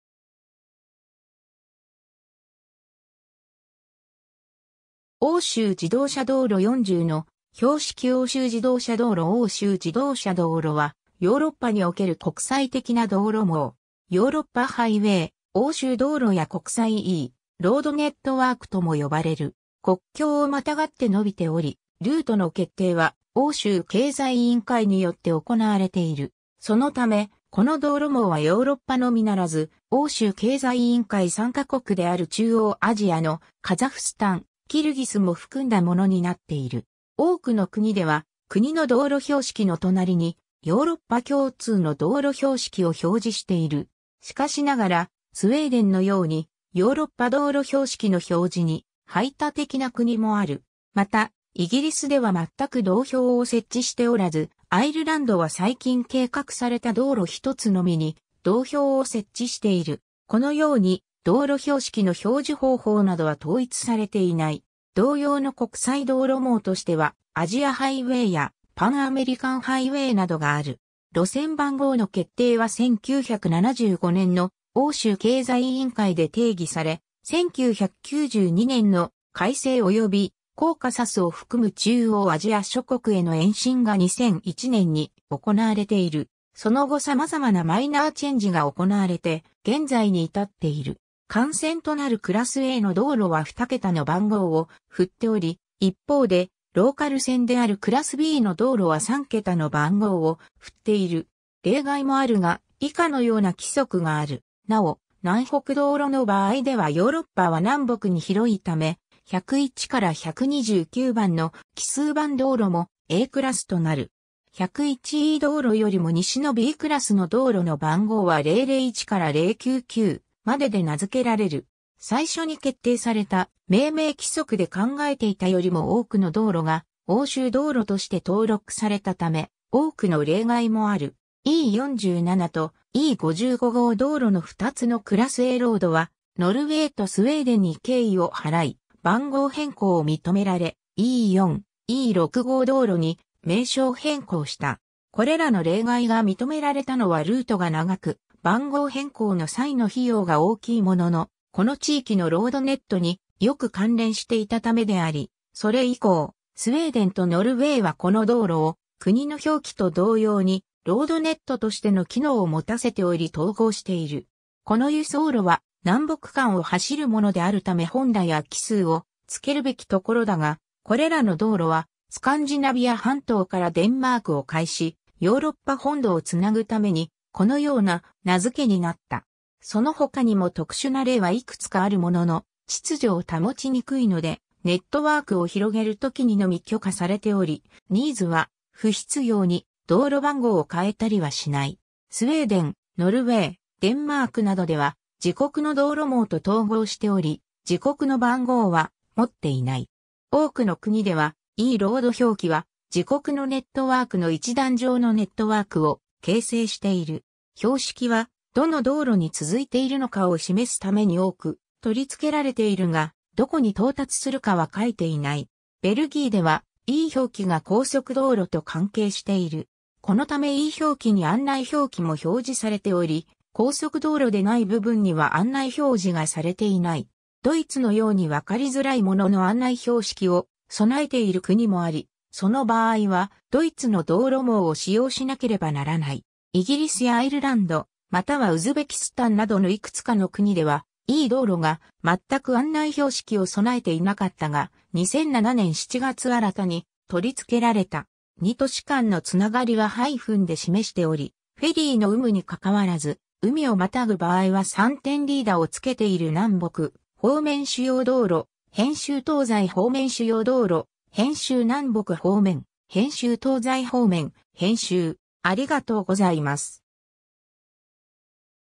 欧州自動車道路
欧州自動車道路40の標識 欧州自動車道路 欧州自動車道路（おうしゅうじどうしゃどうろ）は、ヨーロッパにおける国際的な道路網。
ヨーロッパハイウェイ、欧州道路や国際E-ロードネットワークとも呼ばれる。
 国境をまたがって伸びており、ルートの決定は欧州経済委員会(United Nations Economic Commission for Europe 略称UNECE)によって行われている。
そのため、この道路網はヨーロッパのみならず、欧州経済委員会参加国である中央アジアのカザフスタン、キルギスも含んだものになっている。
 多くの国では国の道路標識の隣にヨーロッパ共通の道路標識を表示している。
しかしながらスウェーデンのように、ヨーロッパ道路標識の表示に排他的な国もある。
また、イギリスでは全く道標を設置しておらず、アイルランドは最近計画された道路一つのみに道標を設置している。
このように道路標識の表示方法などは統一されていない。
 同様の国際道路網としてはアジアハイウェイや、パンアメリカンハイウェイなどがある。
 路線番号の決定は1975年の欧州経済委員会で定義され、1992年の改正およびコーカサスを含む中央アジア諸国への延伸が2001年に行われている。
その後さまざまなマイナーチェンジが行われて(2008年)現在に至っている。
 幹線となるクラスAの道路は2桁の番号を振っており、一方でローカル線であるクラスBの道路は3桁の番号を振っている。
例外もあるが、以下のような規則がある。
 なお、南北道路の場合ではヨーロッパは南北に広いため、101から129番の奇数番道路もAクラスとなる。
101E道路よりも西のBクラスの道路の番号は001から099までで名づけられる。
 最初に決定された命名規則で考えていたよりも多くの道路が欧州道路として登録されたため、多くの例外もある。
 E47とE55号道路の二つのクラスAロードはノルウェーとスウェーデンに敬意を払い、番号変更を認められ、E4、E6号道路に名称変更した。
これらの例外が認められたのはルートが長く、番号変更の際の費用が大きいものの、この地域のロードネットによく関連していたためであり、それ以降、スウェーデンとノルウェーはこの道路を国の表記と同様にロードネットとしての機能を持たせており統合している。
この輸送路は南北間を走るものであるため本来は奇数を付けるべきところだが、これらの道路はスカンジナビア半島からデンマークを介しヨーロッパ本土をつなぐためにこのような名づけになった。
 そのほかにも特殊な例はいくつかあるものの、秩序を保ちにくいので、ネットワークを広げるときにのみ許可されており、UNECEは不必要に道路番号を変えたりはしない。
 スウェーデン、ノルウェー、デンマークなどでは自国の道路網と統合しており、自国の番号は持っていない。
 多くの国ではE-ロード表記は自国のネットワークの一段上のネットワークを形成している。
 標識はどの道路に続いているのかを示すために多く取り付けられているが、どこに到達するかは書いていない。
 ベルギーではE-表記が高速道路と関係している。
このためE表記に案内表記も表示されており、高速道路でない部分には案内表示がされていない。
 ドイツのようにわかりづらいものの案内標識を備えている国もあり、その場合はドイツの道路網を使用しなければならない。
イギリスやアイルランド、またはウズベキスタンなどのいくつかの国では、E-道路が全く案内標識を備えていなかったが、2007年7月新たに取り付けられた。
 二都市間のつながりはハイフンで示しており、フェリーの有無にかかわらず海をまたぐ場合は三点リーダをつけている 南北方面主要道路[編集] 東西方面主要道路[編集] 南北方面[編集] 東西方面[編集]